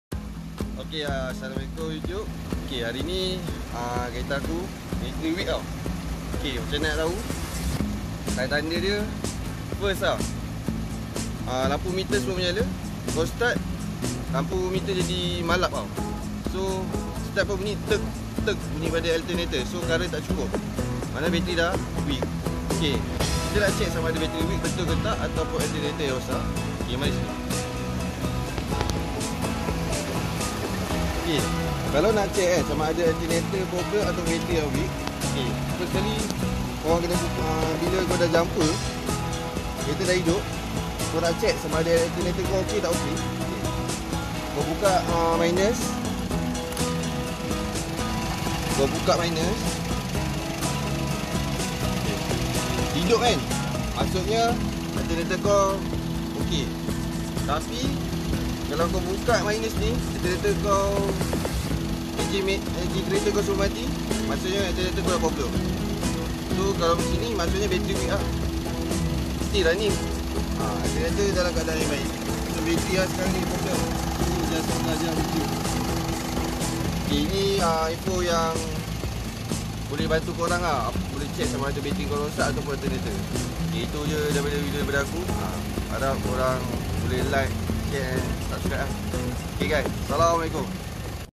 CARA NAK PERIKSA SAMADA BATERI ATAU ALTERNATOR YANG MASALAH
Video ni aku akan kongsikan utk korang, cara yg mudah dpt membantu korang utk check kereta korang yg mana samada bateri atau alternator korang yg kong. Harap dapat membantu korang.